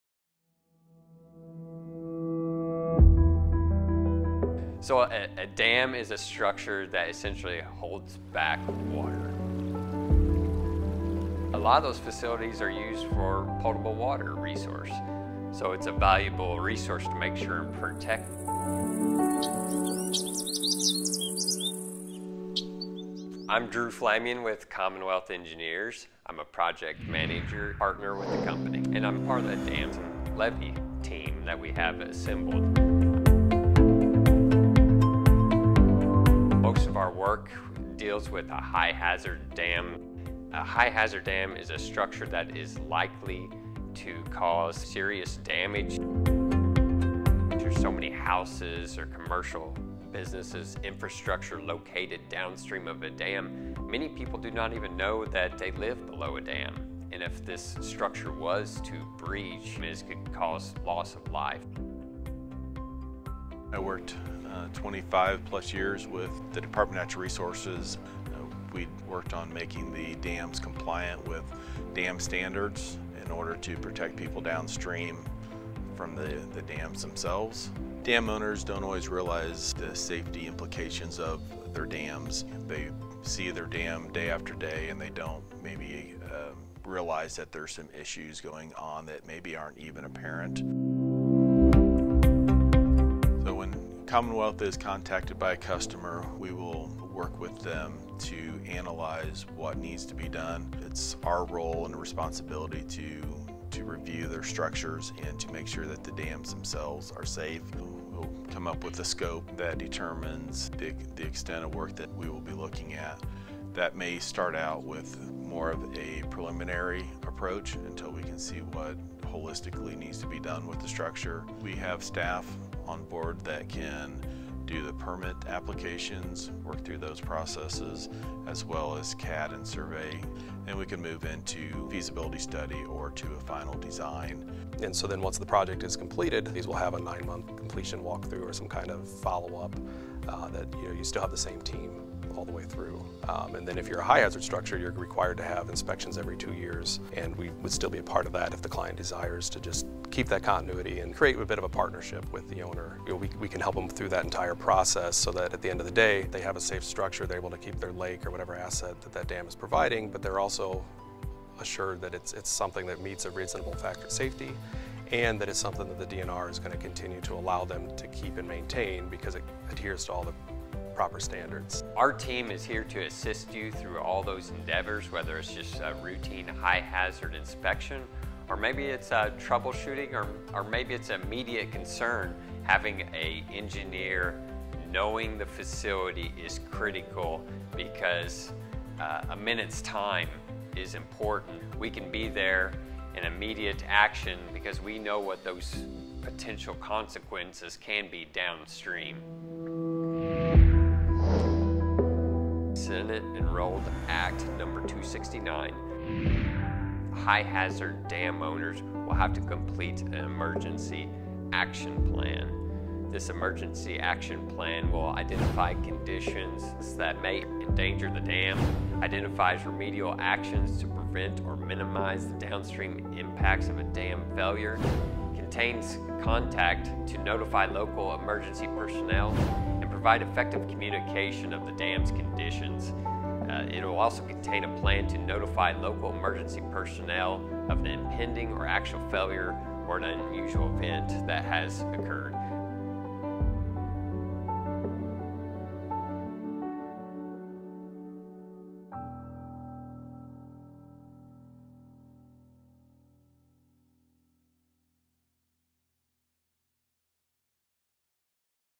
Dams & Levees
Commonwealth Engineers' Dams and Levees Team provides inspections, evaluations, condition assessments, designs, and construction services for high-hazard dams.